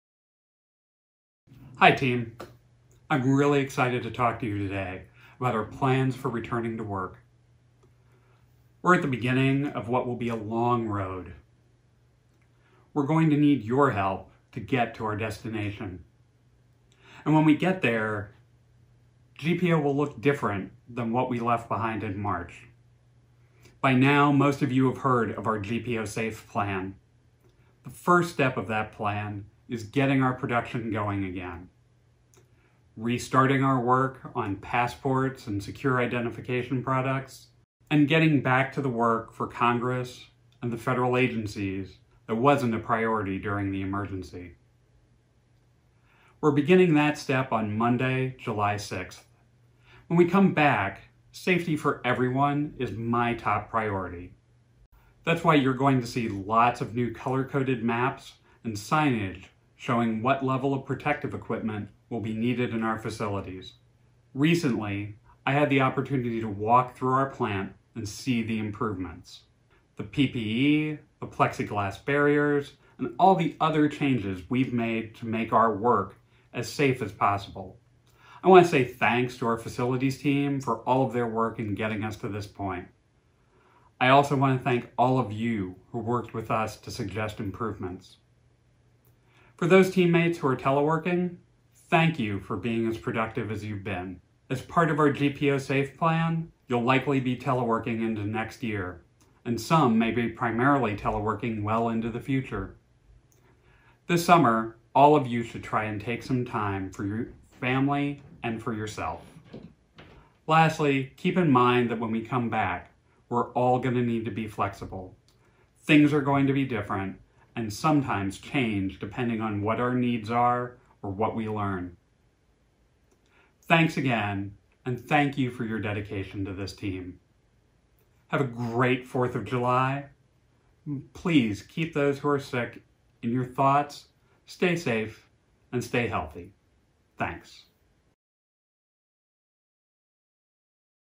GPO Director Hugh Halpern Introduces GPOSAFE Plan
GPO Director Hugh Halpern discusses GPOSAFE, GPO's plan to return to work safely. The plan offers a holistic approach to working in the era of COVID–19 and beyond.